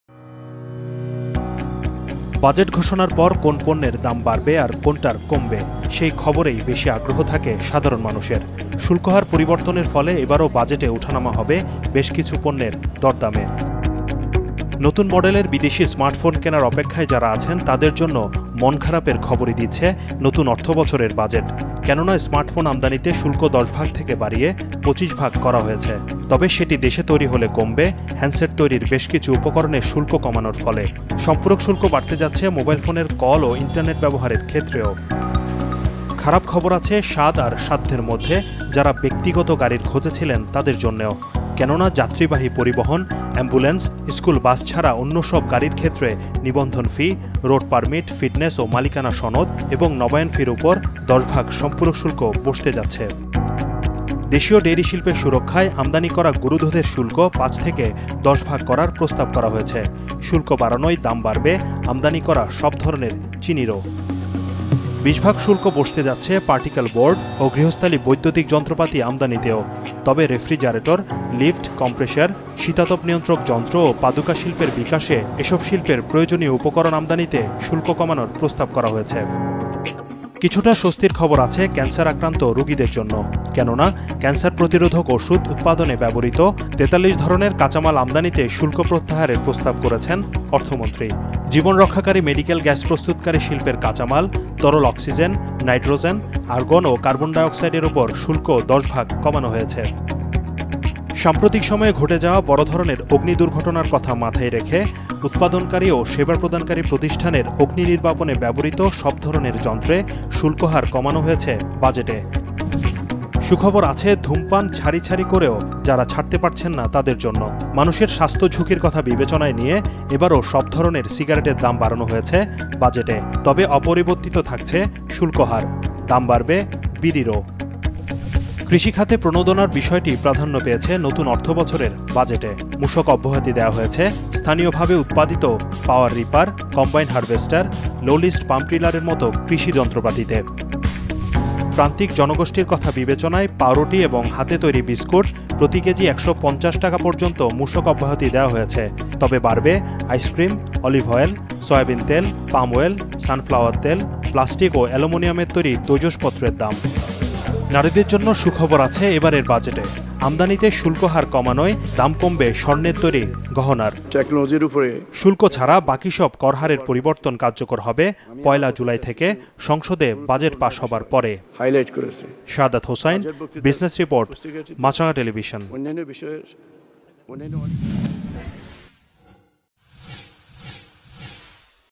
দাম বাড়লো কিসে? কমলো কার? | Bangla Business News | Business Report 2019
বাজেট ঘোষণার পর কোন পণ্যের দাম বাড়বে আর কোনটার কমবে সেই খবরেই বেশি আগ্রহ থাকে সাধারণ মানুষের। শুল্ক হার পরিবর্তনের ফলে এবারও বাজারে উঠা নামা হবে বেশ কিছু পণ্যের দরদামে। ২০১৯-২০ অর্থবছরের প্রস্তাবিত বাজেটে কোন পণ্যের দাম বাড়লো আর কোন পণ্যের দাম কমলো সেটা জানা যাক এবার...